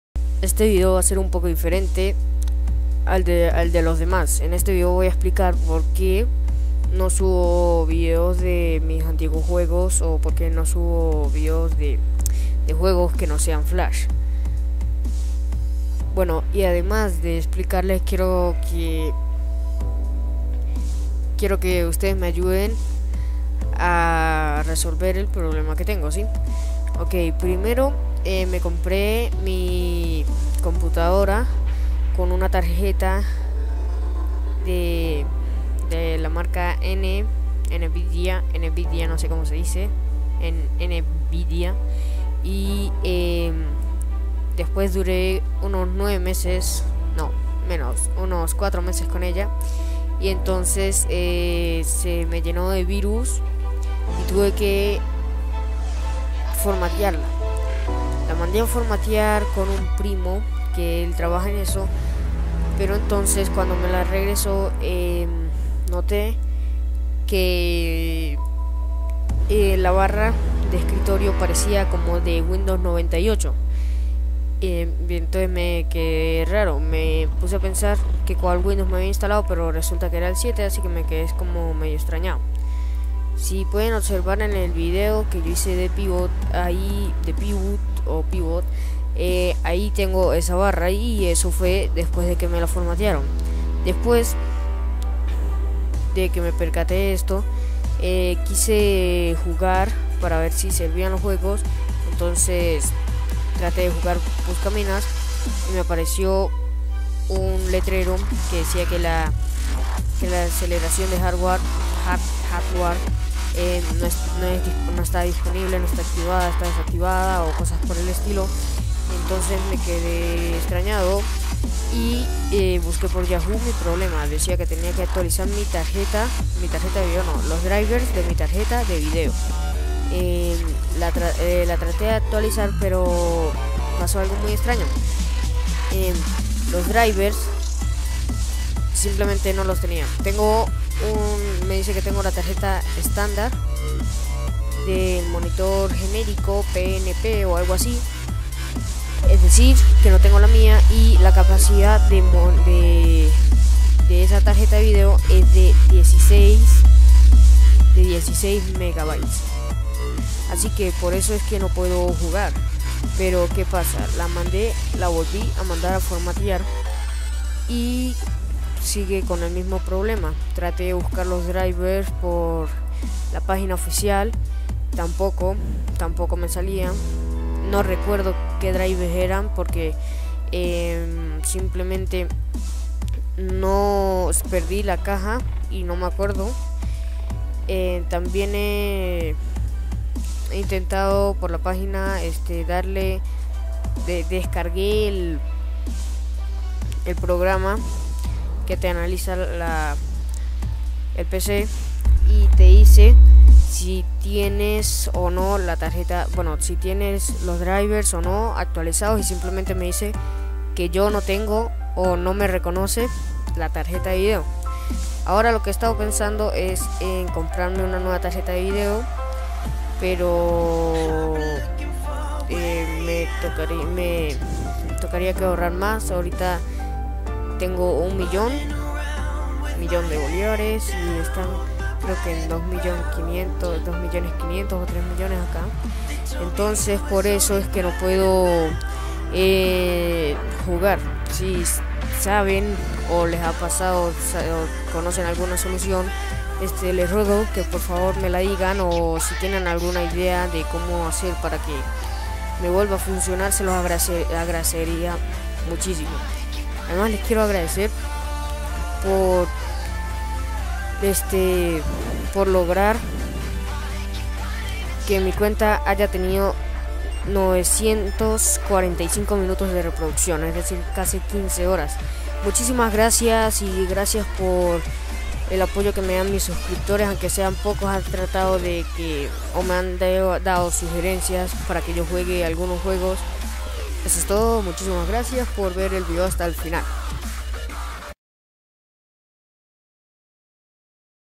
¡Necesito tu ayuda!- TerkozDark
¿Hola chicos y chicas como estan?, aqui en este vidoe les vengo a pedir su ayuda xD, si se que youtube no es Yahoo pero se los pido aun así xD.

Ademas gracias por hacerme llegar hasta las 600 visitas y ademas tener 953 minutos de reproduccion y todo eso gracias a ustedes :D